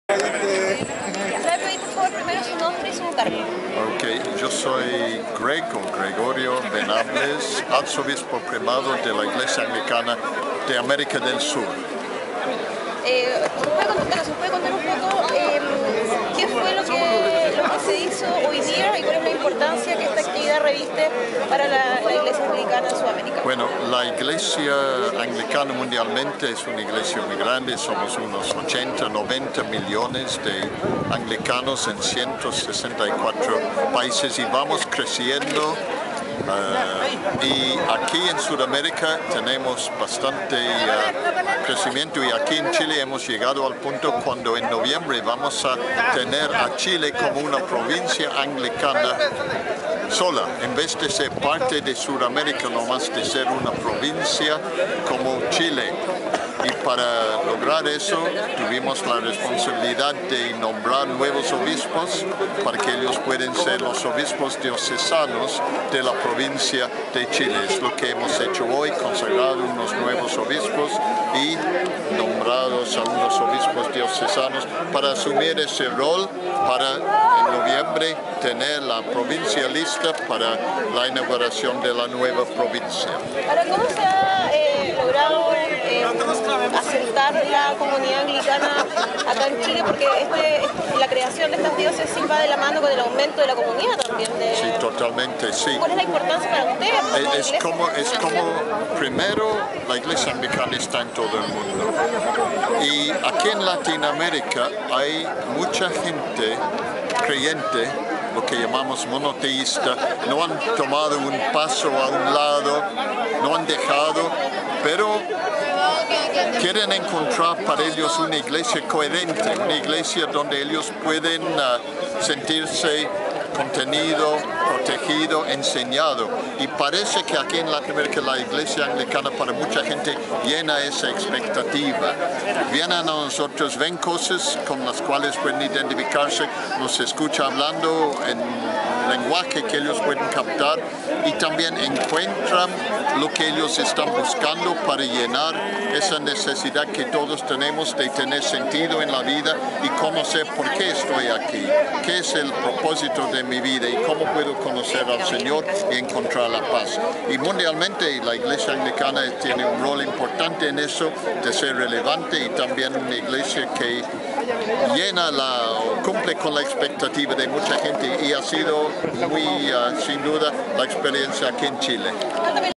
entrevista Arzobispo primado greg Venables
El 15 de Julio del 2018, a las 11:00 Hrs. en la Catedral Anglicana de Chile, St. Paul's, del Cerro Concepción de Valparaiso, con la Asistencia de la comunidad anglicana de Chile, sus Pastores y presidida por el Embajador Británico en Chile Sr. Jamie Bowden, el Primado de la Provincia Anglicana de Sud America Rvmo. Greg Venables y el Arzobispo Diocesano Rvmo. Hector Zavala; se preocedio a la Consagración como Obispos a los Hermanos: Samuel Morrison y Enrique Lago, asimismo se hizo un reconocimiento como Obispos Diocesanos a los Obispos: Lago, Morrison y Abelino Apeleo.